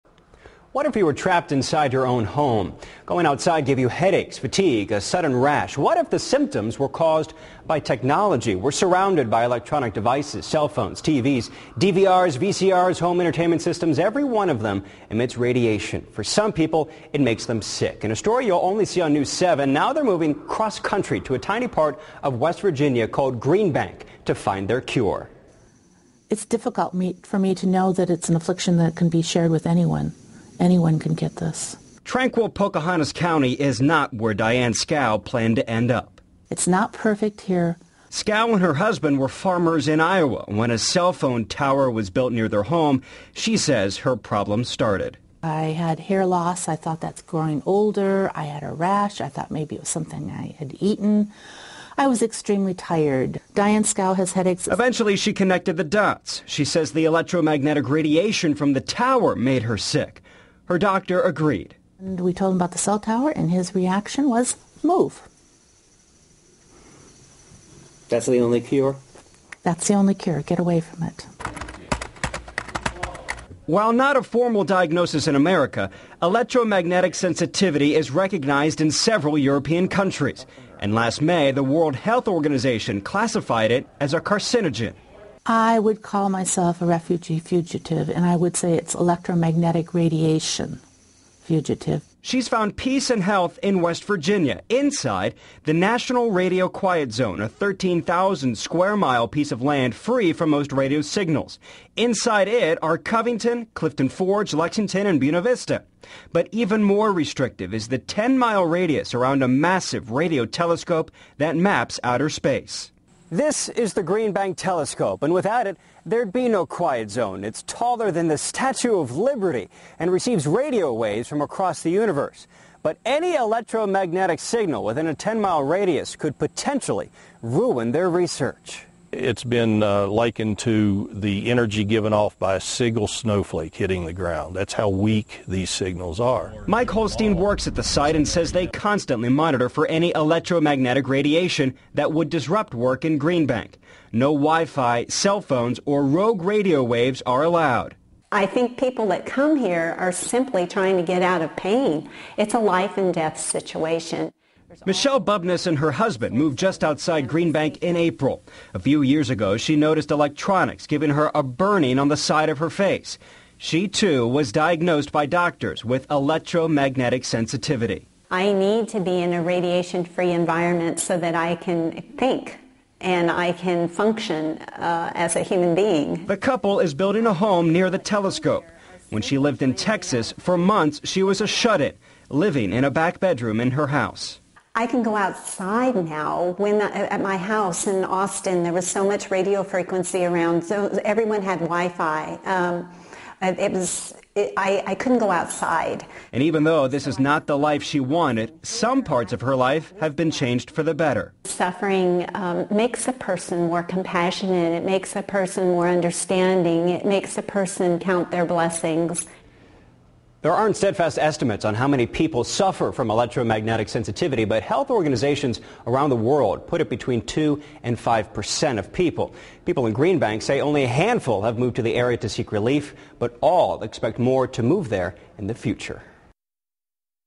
Wi-Fi Refugees move to Green Bank to escape electromagnetic radiation
"Wi-Fi Refugees" moving to Green Bank, West Virginia to escape electromagnetic radiation. A massive radiotelescope prohibits any mainstream electronics from being used nearby.
As seen on WDBJ7.com